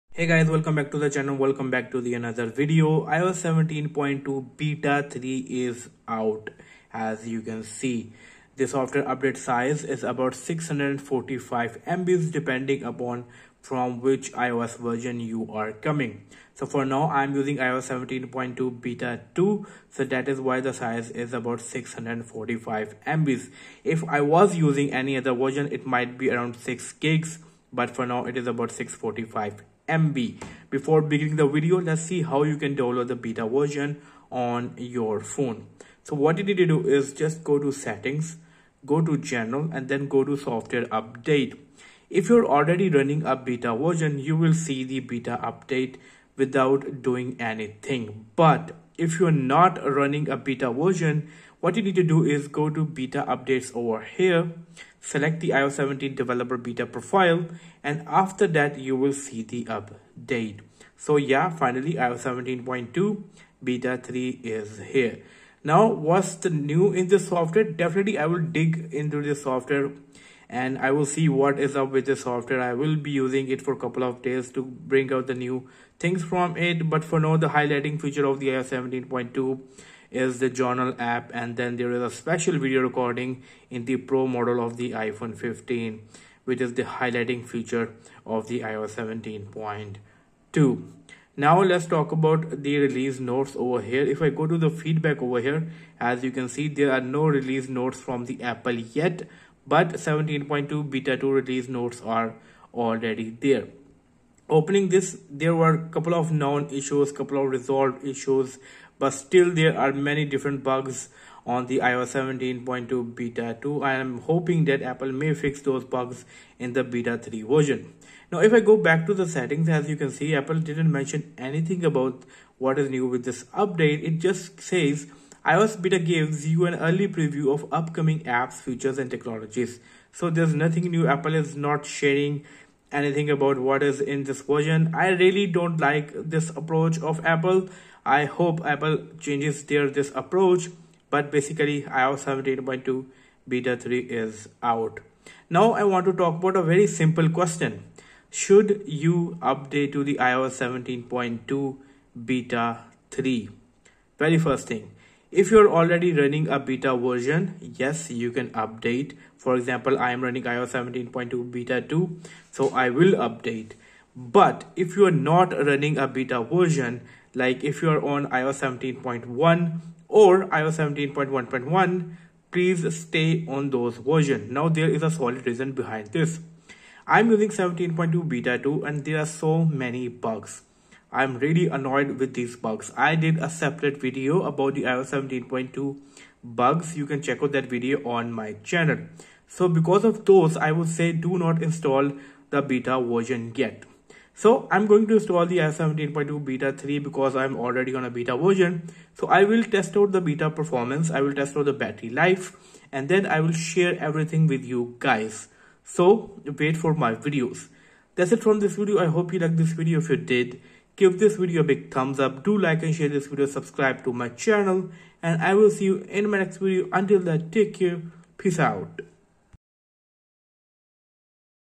iOS 17.2 Beta 3 is Out - Should You Update?
Hey, Guys, I am your iOS Guy who is here to share his experience with iOS and iPhone.

Apple releases iOS 17.2 Beta 3 and in this video, I am going to talk about this update.
This video is going to be about iOS 17.2 Beta 3 Should You Update?
In this video, I will discuss iOS 17.2 Beta 3 What's New, iOS 17.2 Beta 3 New Features, and More.
Just after a week of Beta 2, iOS 17.2 Beta 3 Update is out.